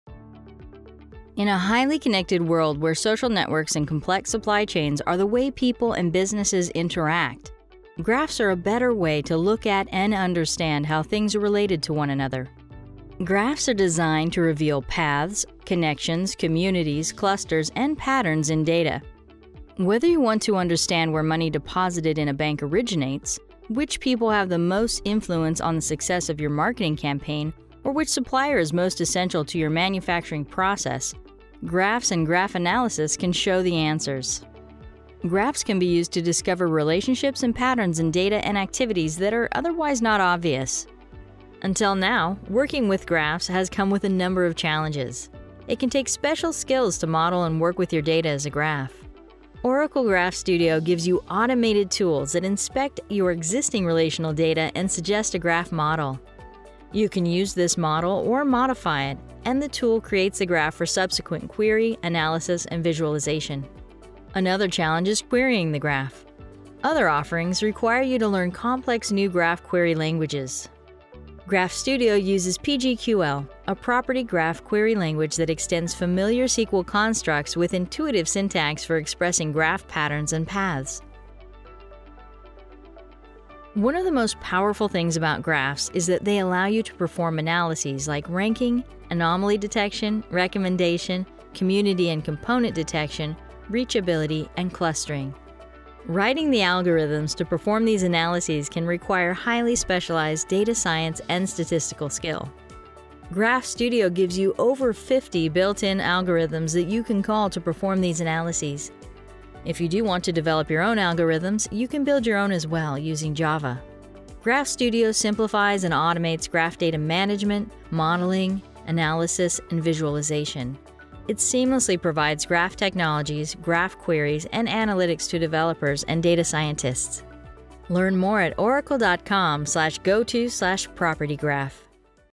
Simplify Graph Analytics with Autonomous Database
Graphs can be used to discover relationships and patterns in data and activities that are otherwise not obvious. Oracle Graph Studio simplifies and automates graph data management, modeling, analysis, and visualization. It seamlessly provides graph technologies, graph queries and analytics to developers and data scientists.